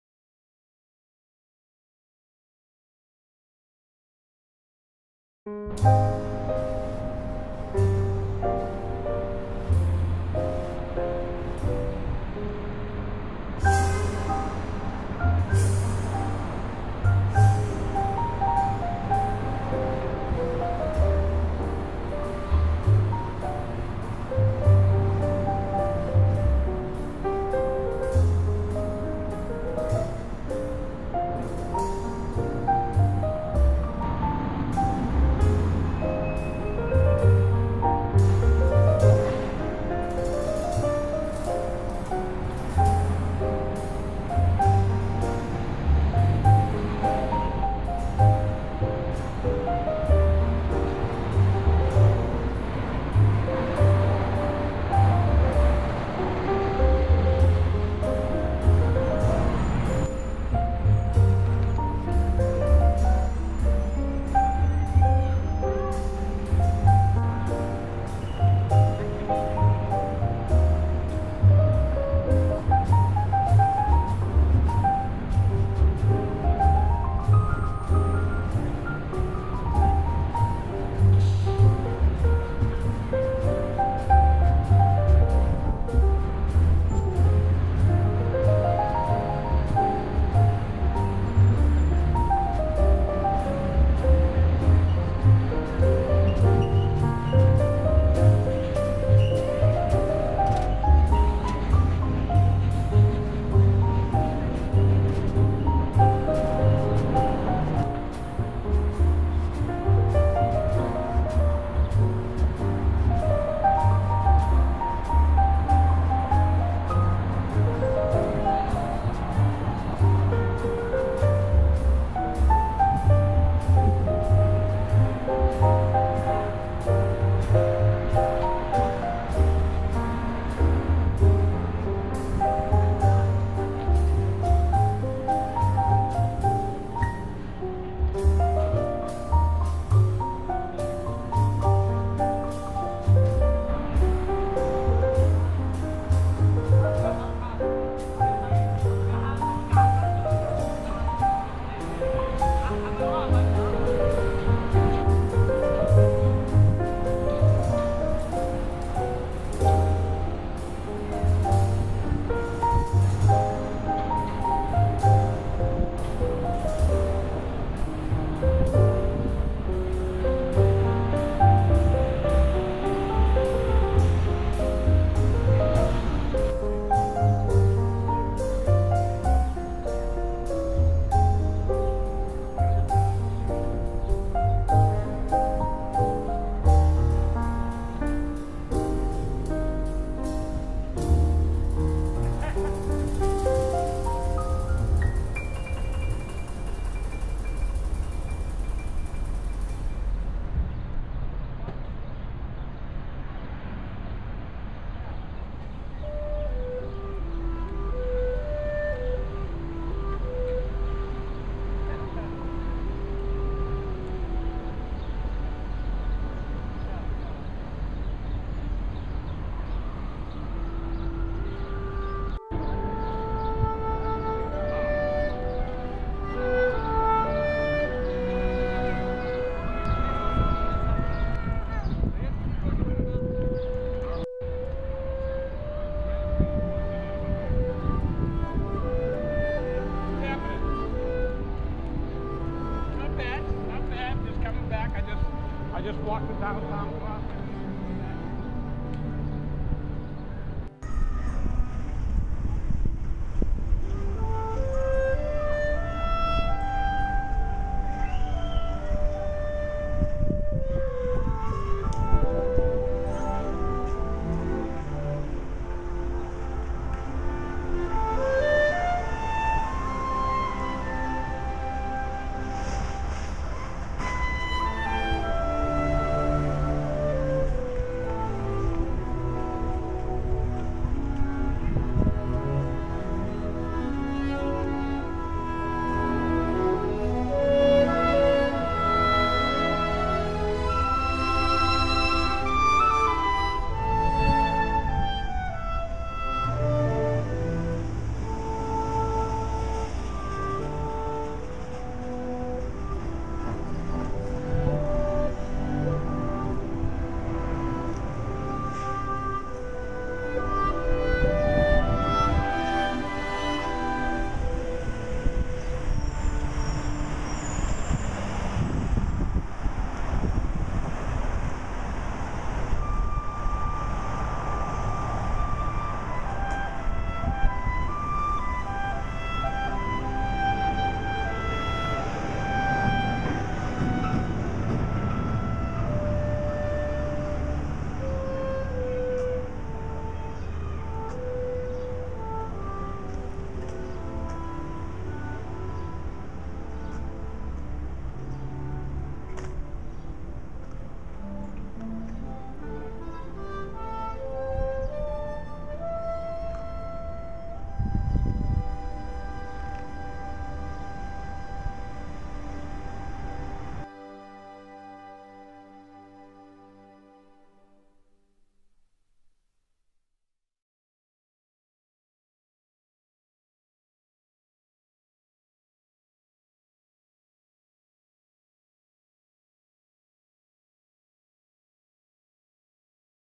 Boston City 2 by Peter Wong.wmv
Short movie, City of Boston, is made by Peter Wong, a 72 years old senior who is the student in Castle Square Technology Center Adult Technology Program. video taping, eding and music are all by himself, and he has studied coputer in CSTC for more than 3 years.
Now he can browing on line, for the Chinese local news and type up a long article in Microsoft Word.